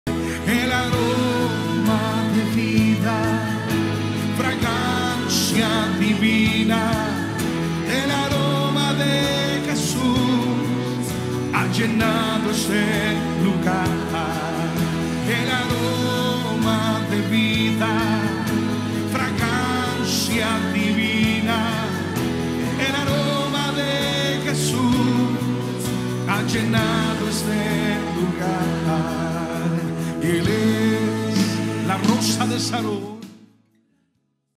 El Aroma de Vida...
Deja que el aroma de Jesús inunde diariamente cada área de tu vida. Deja que su dulce fragancia toque tu matrimonio, familia, estudios y ministerio. Gracias a nuestro amigo y pastor Ericson Alexander Molano  por dirigirnos en adoración en nuestra Worship Session